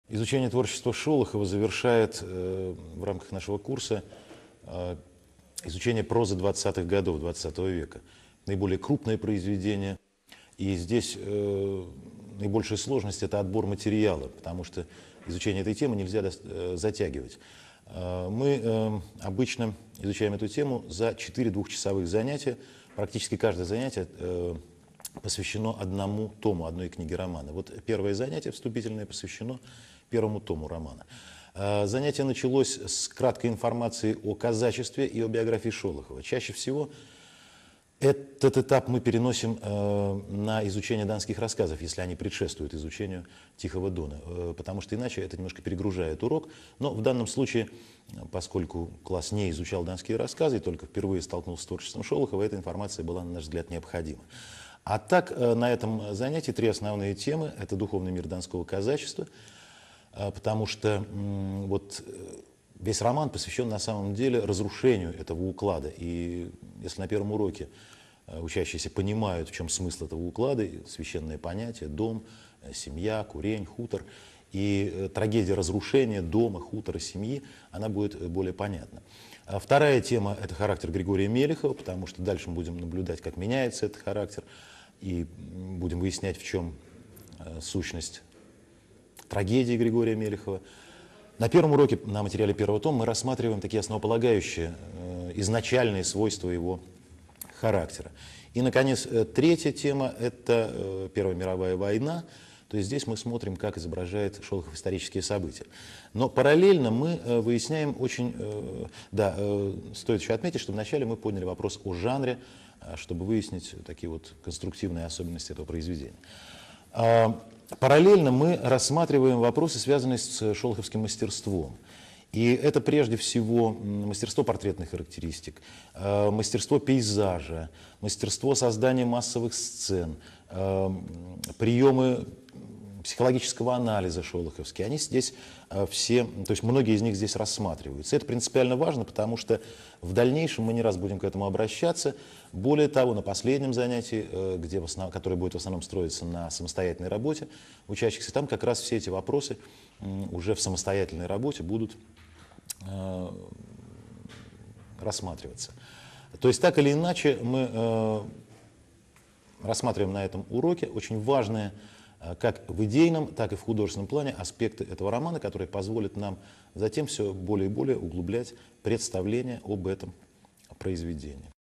Организация урока «Изучение творчества М. Шолохова на примере романа «Тихий дон» (М.А,Нянковский)
Об особенностях организации урока литературы в 11 классе «Изучение творчества М. Шолохова на примере романа «Тихий дон» рассказывает Нянковский Михаил Александрович,учитель МОУ "Провинциальный колледж" г. Ярославля.  Победитель конкурса “Учитель года России 1994”.